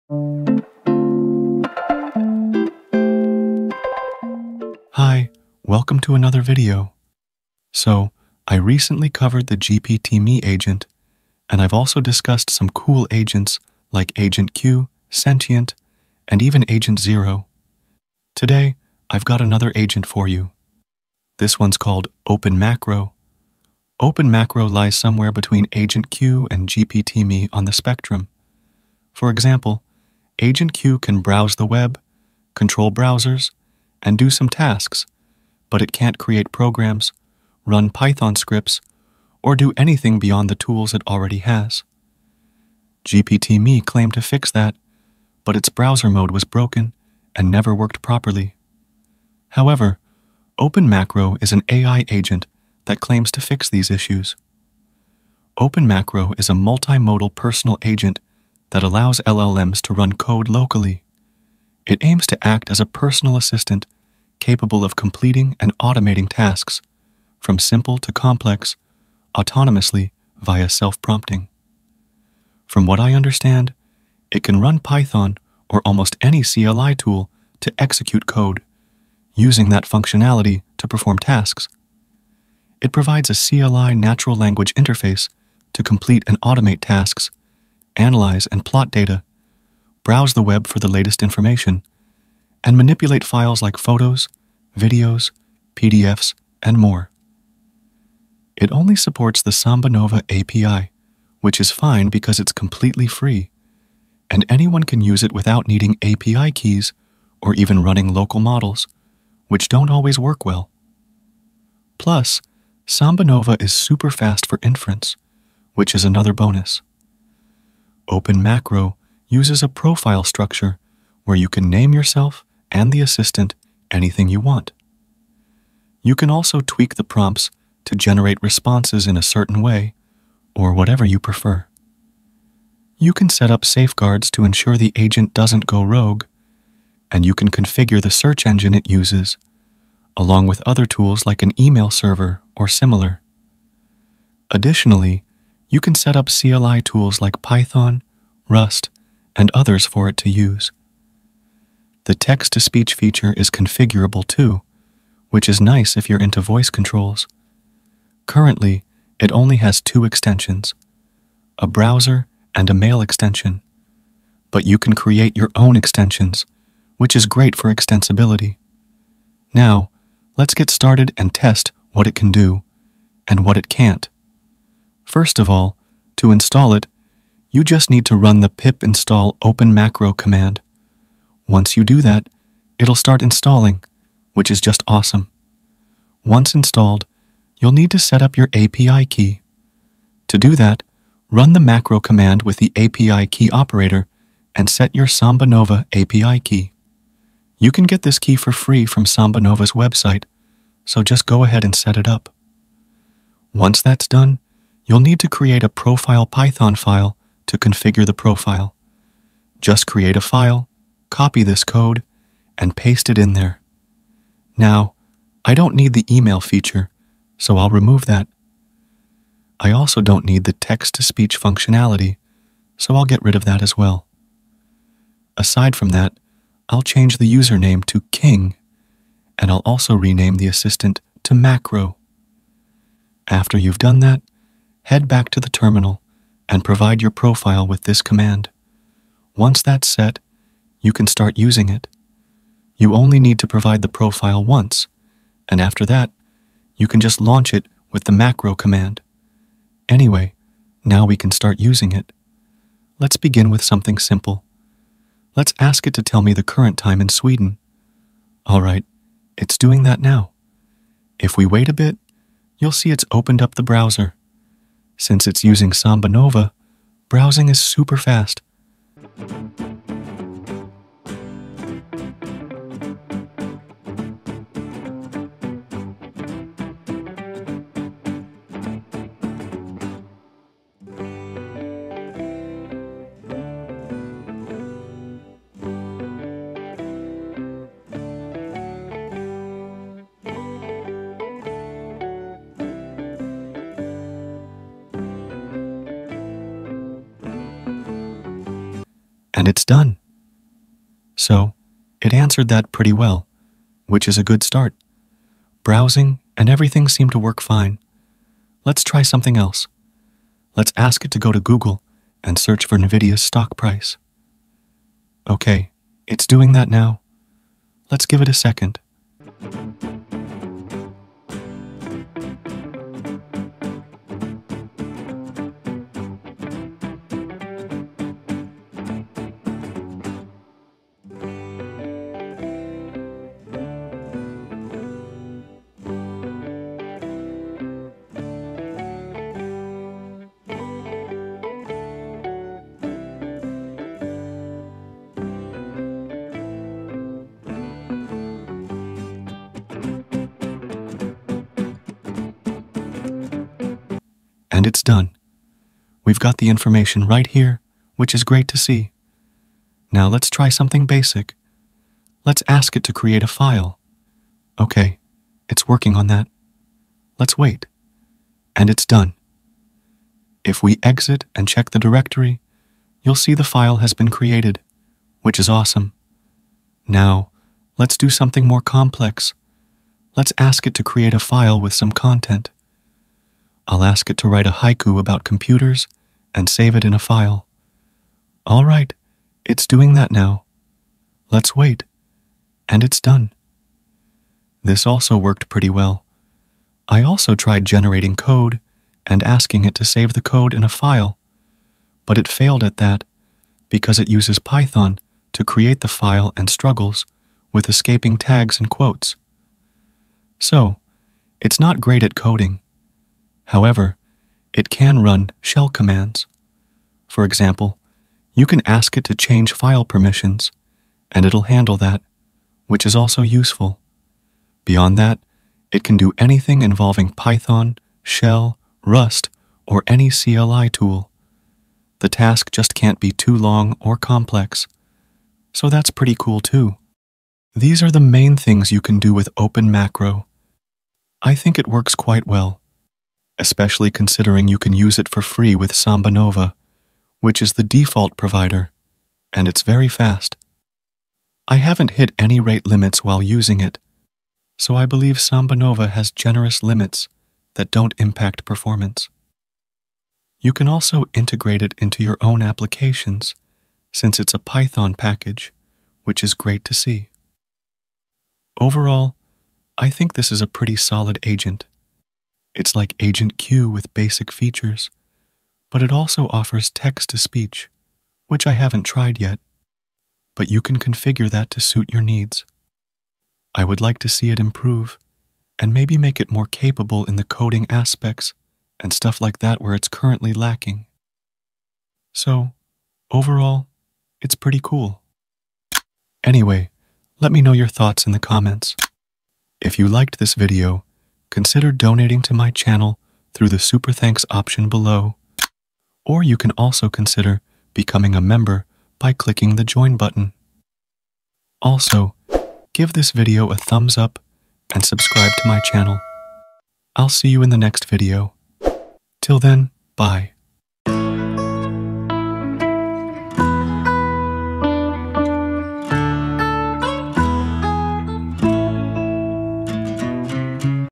Agent-OpenMacro : This SELF-PROMPTING AI Agent CAN DO ANYTHING & CONTROL YOUR BROWSER (Code,RAG,etc)
In this video, I'll be talking about OpenMacro, which is a new AI Agent Framework that can do Anything & Everything. It can also control your whole Browser & Computer. It can do Coding, RAG, Research, Text-To-Application, Text-To-Frontend, RAG, etc.  It is an All-In-One & AI Agent that is fully opensource and local. I'll be trying it out and testing it to find out. If it's really that good.  I'll be generating some simple applications but you can also use it to generate Games, Applications, Web Applications, Websites, Frontend, Backend and multiple other things. You can also do Text-To-Frontend, Text-To-Application, Text-To-Game and other things with this. MicroAgent can also be used with any opensource LLM, OpenAI models or the other Claude models such as Llama-3.1, GPT-4O, Claude-3, CodeQwen, Mixtral 8x22b, Mixtral 8x7b, GPT-4, Grok-1.5 & Gemini Code Assist.

Key Takeaways:

🎯 Discover OpenMacro: Your New AI Assistant – Dive into the world of AI agents with OpenMacro, a powerful tool that bridges the gap between Agent Q and GPT-Me for seamless task automation and code execution.

💻 Run Code Locally with Ease – OpenMacro allows you to run Python scripts, CLI tools like Rust, and more—turning natural language commands into actionable tasks that streamline your workflow.

🚀 Fast and Free with SambaNova API – OpenMacro supports the free SambaNova API, delivering lightning-fast performance without the need for complex API keys or local models, making it super easy to use for everyone.

🛠️ Extensibility at Its Core – OpenMacro gives you the freedom to create custom extensions, from browser integrations to email management, ensuring that your AI agent evolves with your needs.

🧠 Highly Configurable Profiles – Personalize your AI assistant with custom profiles, tweak response styles, and configure everything from text-to-speech to search engines—giving you full control over your AI experience.

📂 Simplify File Management – Whether you need to create files, change permissions, or even write haikus, OpenMacro can handle it all, making file management a breeze with simple commands.

📈 Perfect for Developers and Tech Enthusiasts – OpenMacro is a developer’s dream, running everything from shell commands to Python code—perfect for automating tasks and integrating it into your applications.

Timestamps:

00:00 - Introduction
00:22 - About OpenMacro
02:58 - Usage & Testing
08:15 - Ending